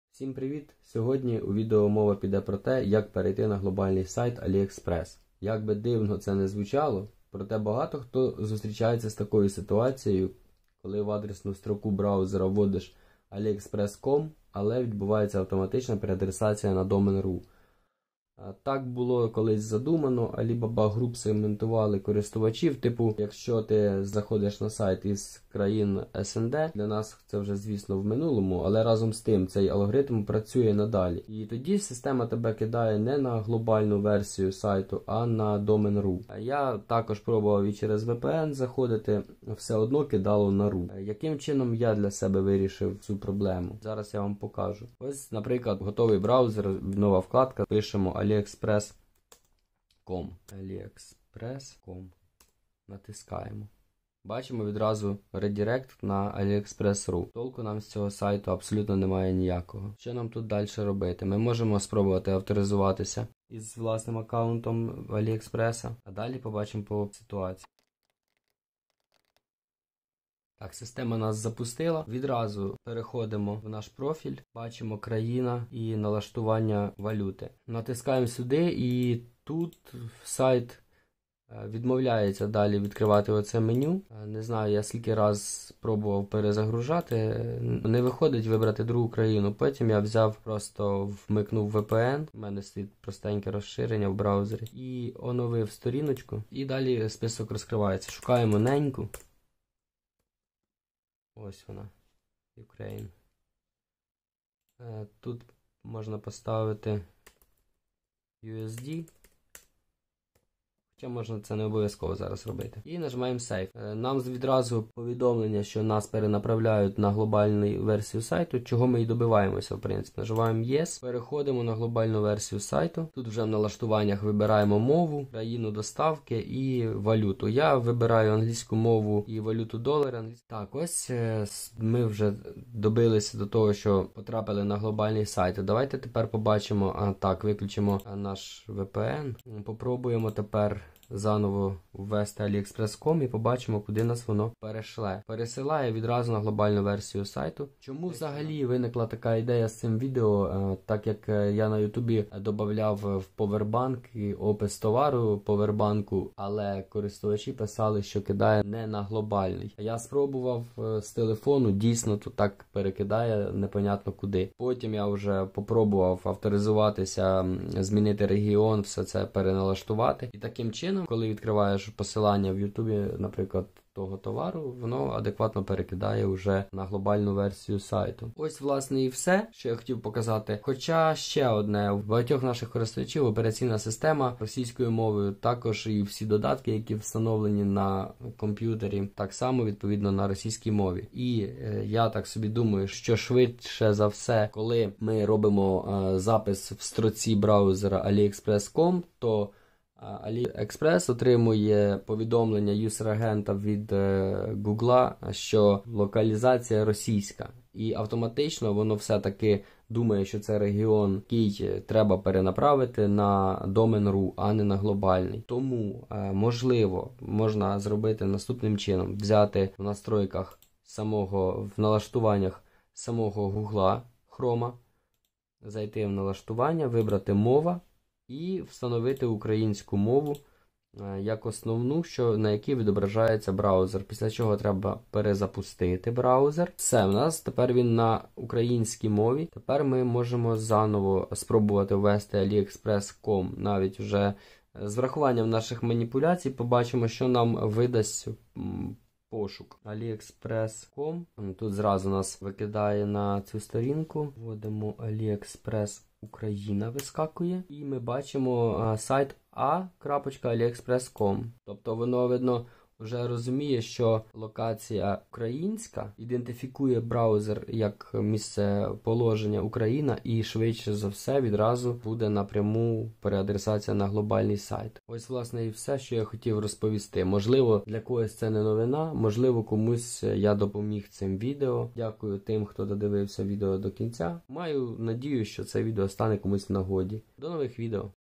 Як отримати доступ до AliExpress.com з України за декілька хвилин?
Що робити, коли вас вперто переадресовують не на aliexpress.com?
є спосіб - почистити файли cookie цього сайту в браузері, але я спробував альтернативні варіанти, про них йдеться на відео.
Приємного перегляду ( чи, скоріше за все - прослуховування :) )